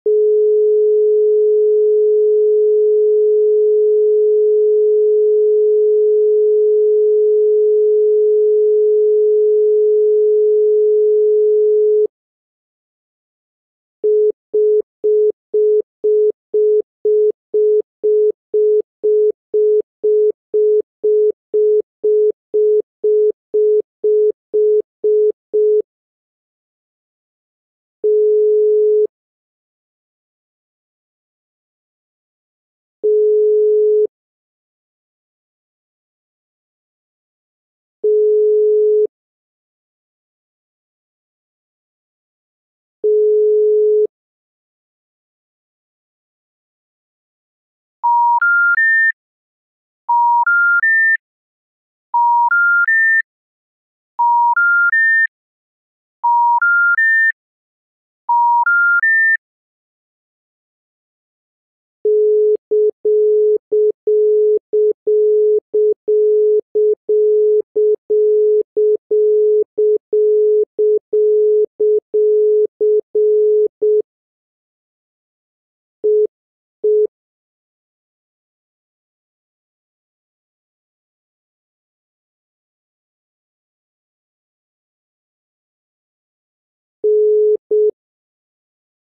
Phone sounds: Mexico. ALL THE TONES! (Dial, busy/reorder, ringback, SIT, call waiting, and more)
TONZ from Around the World: MEXICO

Timeline breakdown

0:00 - 0:12 : Dial
0:14 - 0:26 : Busy/Reorder/Congestion
0:28 - 0:48 : Ringback
0:48 - 1:00 : SIT Special Information Tone (CSCO)
1:02 - 1:16 : Intervention (CSCO)
1:16 - 1:27 : Call Waiting (CSCO)
1:27 - 1:29 : Warning - Operator Intervening (ITU-ETSI).

Dial tone: 425 Hz, continuous.
Busy/Reorder/Congestion tone : 425 Hz, 0.25 s on 0.25 s off.
Ringback tone: 425 Hz, 1.0 s on 4.0 s off.
SIT (CSCO): (950 Hz) 0.33 s on 0.03 s off (1400 Hz) 0.33 s on 0.03 s off (1800 Hz) 0.33 s on 1.0 s off.
Intervention tone (CSCO): 425 Hz, 0.5 s on 0.17 s off 0.17 s on 0.17 s off.
Call waiting tone (CSCO): 425 Hz, 0.2 s on 0.6 s off 0.2 s on 10 s off.
Warning tone (ITU-ETSI): 425 Hz, 1×(0.5 s on 0.17 s off 0.17 s on 0.17 s off).